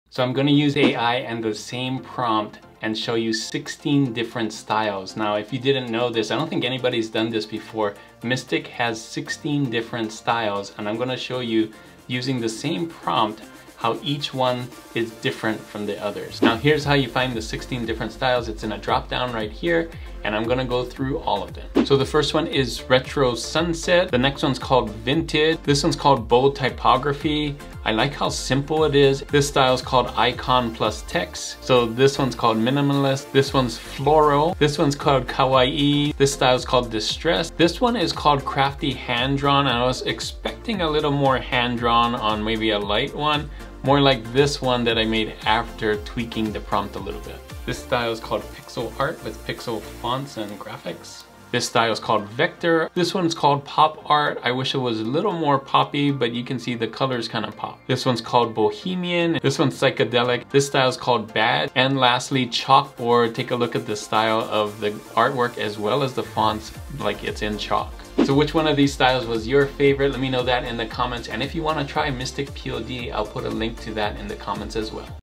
See All 12 Mystic Styles with The Same Prompt
Did you know Mystic POD has 12 different preset styles?
Here is what they do and how they are different.

Elgato Key Light
LG 38" UltraWide Monitor